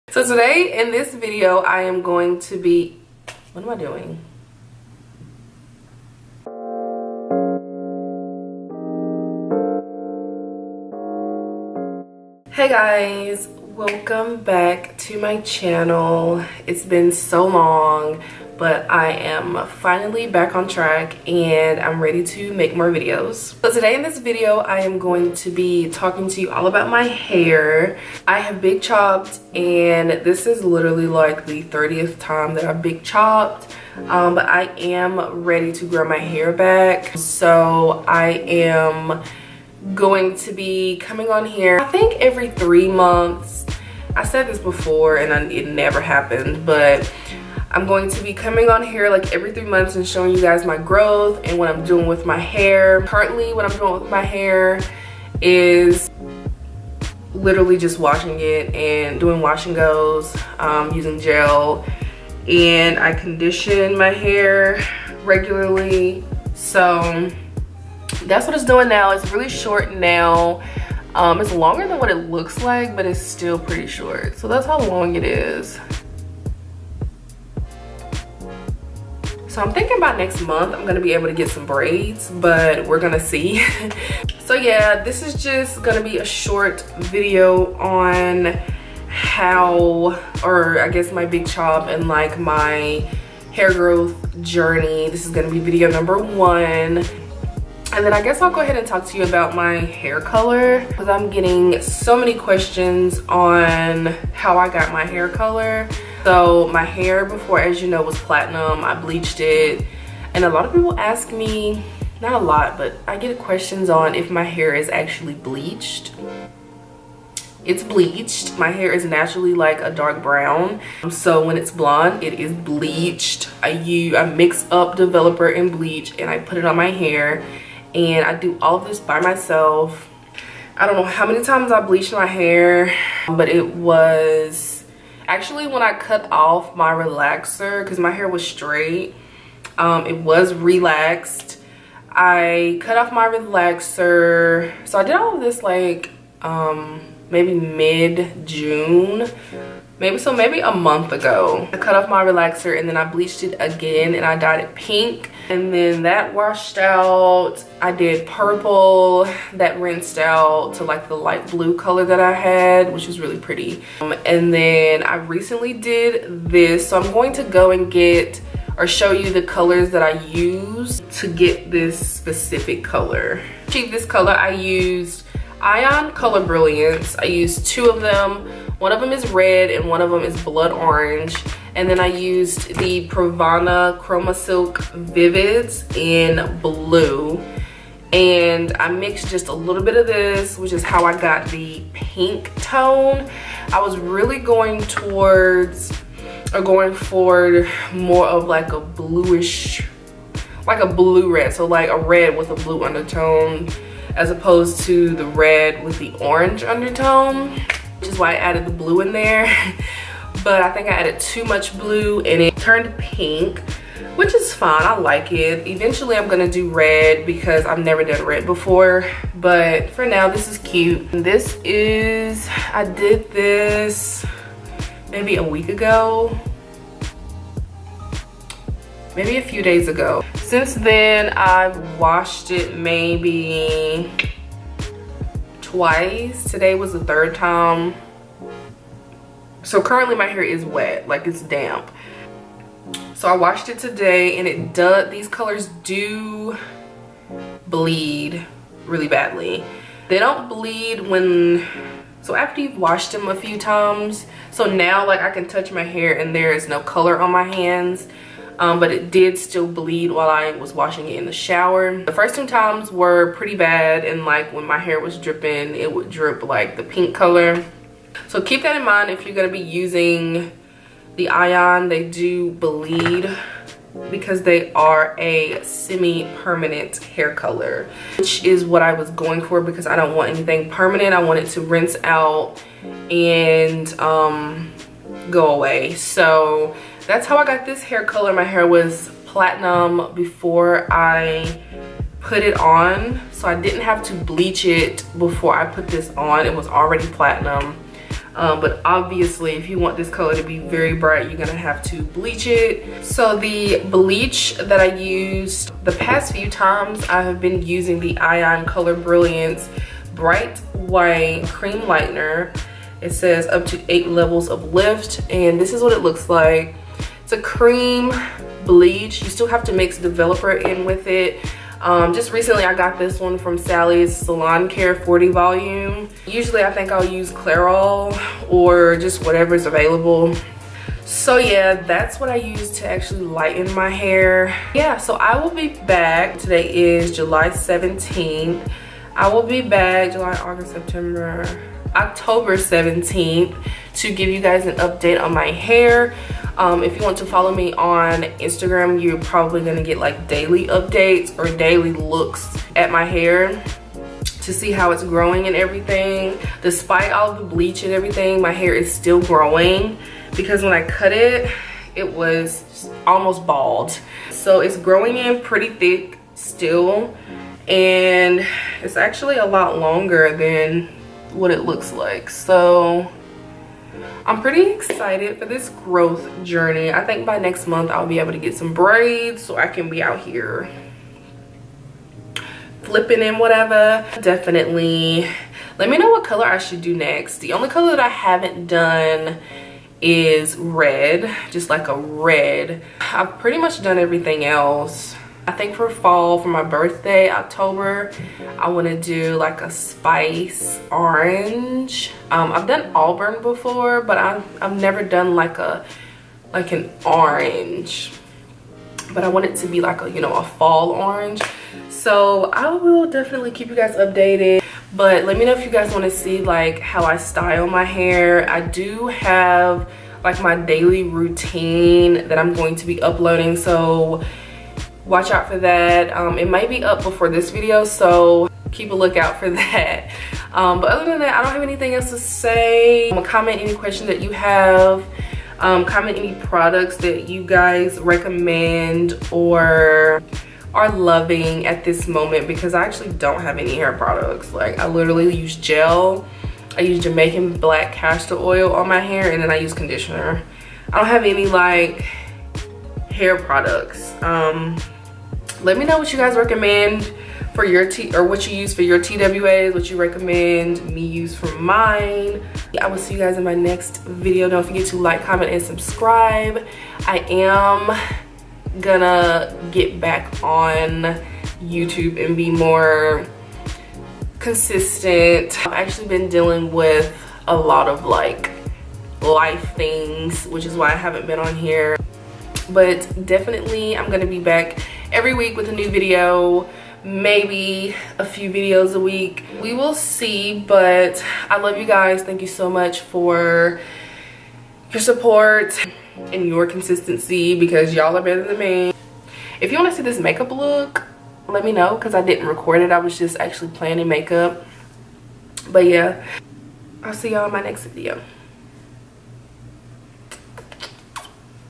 Natural Hair Journey | Big Chop + Lets Talk Color
Hey y’all, I’m back with another video. This time I’m talking all about my hair! I’m growing my hair back out,  for real this time.

PRODUCTS MENTIONED

ION COLOR BRILLIANCE BLOOD ORANGE/RED
ION COLOR BRILLIANCE BRIGHT WHITE CREAM BLEACH
SALON CARE 40V DEVELOPER
PRAVANA CHROMASILK VIVIDS BLUE

YOUTUBER LOOKING FOR EXTRA CASH OR FREE PRODUCTS?